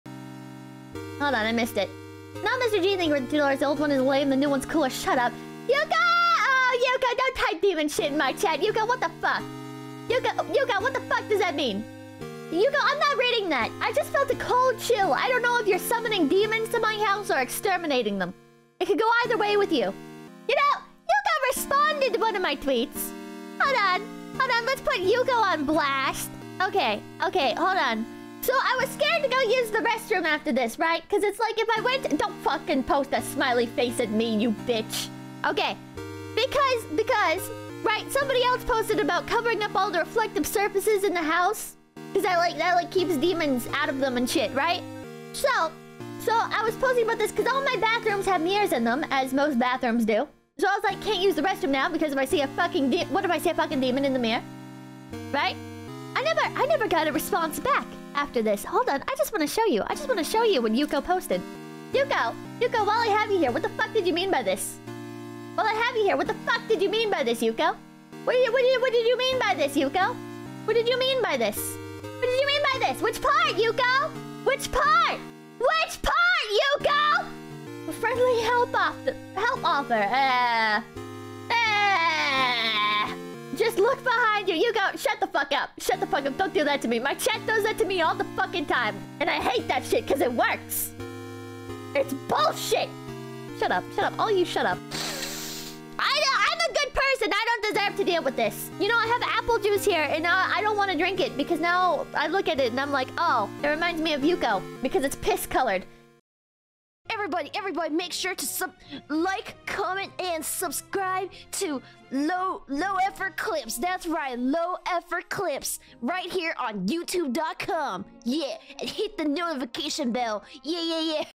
Pippa gets help
she didn't went to the bathroom in 4 days